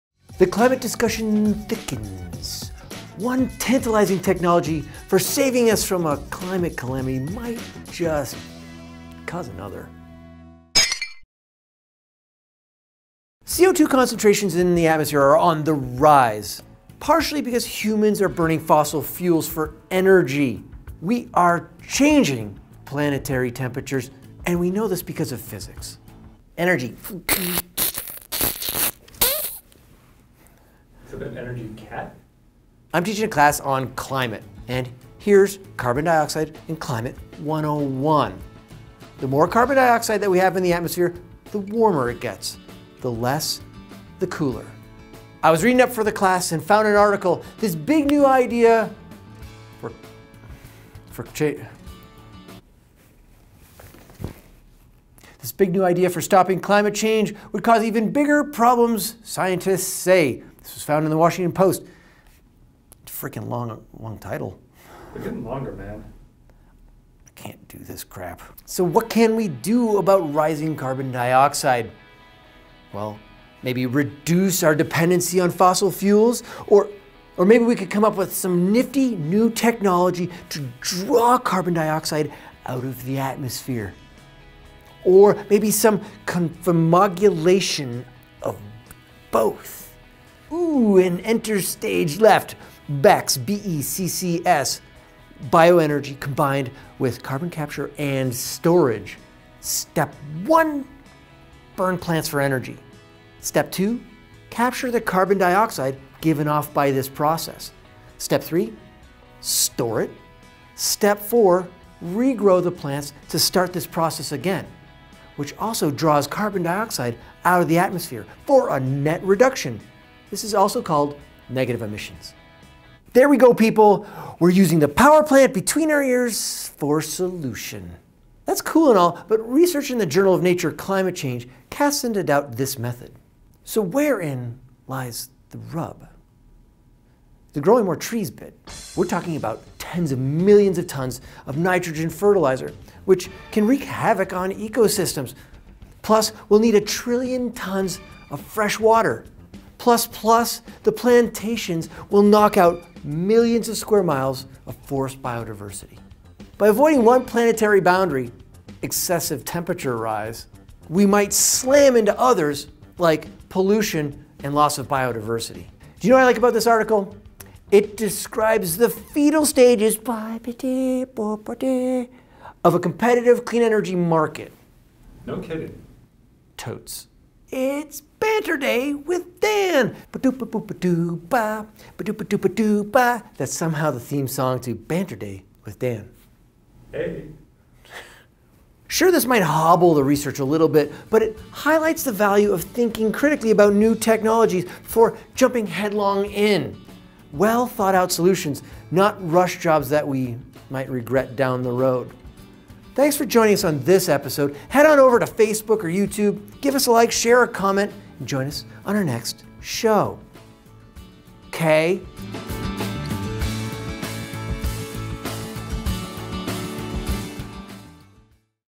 Why Bioenergy Isn't Ready to Save the Planet
Bioenergy with Carbon Capture and Storage (BECCS) is a promising technology to help us in our pursuit of generating cleaner energy.  There are still some kinks to iron out, though.  Come explore BECCS with us a bit more in this latest video.

You can also break science with us on other social media sites.